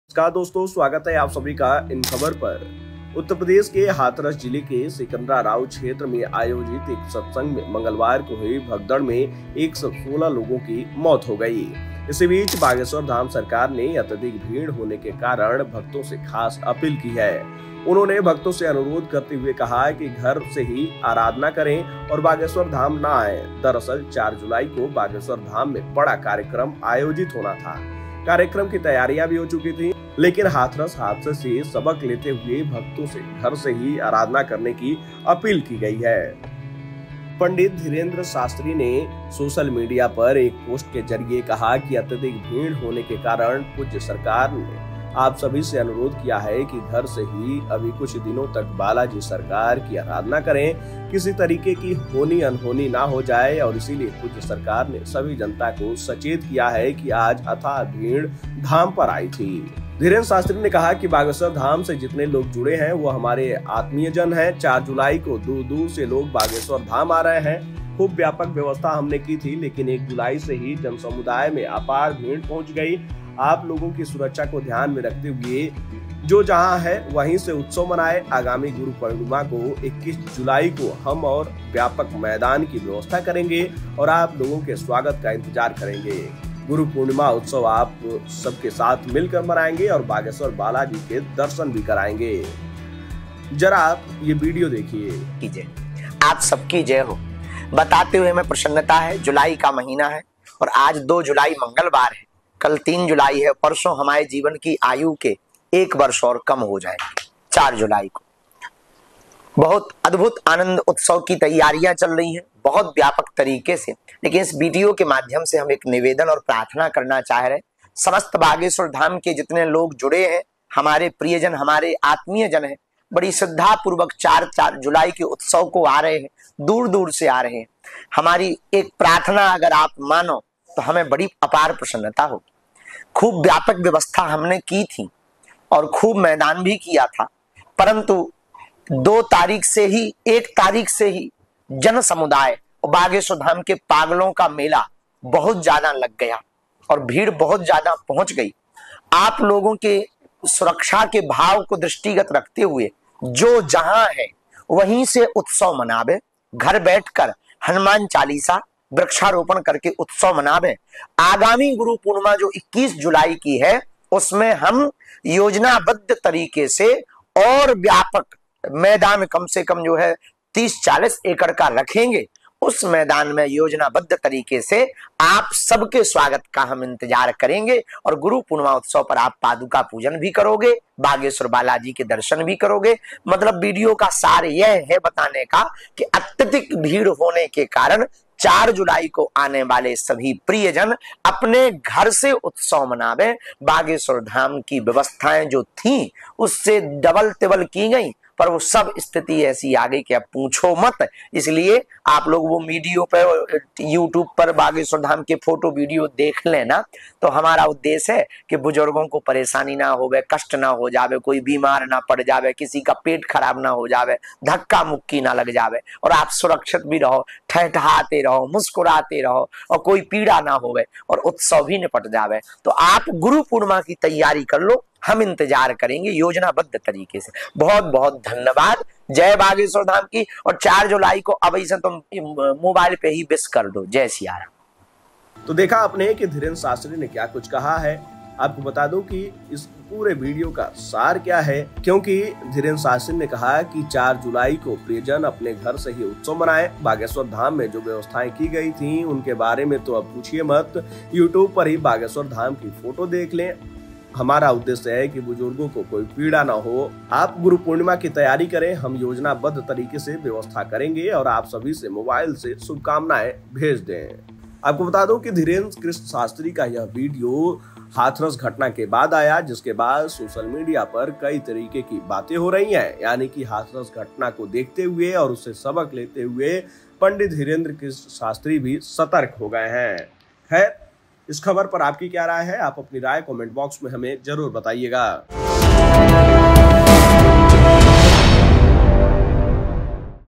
UP News: हाथरस की घटना के बाद बाबा बागेश्वर को सताने लगी चिंता, भक्तों से की बड़ी अपील | Inkhabar
UP News: हाथरस की घटना के बाद बाबा बागेश्वर को सताने लगी चिंता, भक्तों से की बड़ी अपील

UP News: After the Hathras incident, Baba Bageshwar started worrying, made a big appeal to the devotees.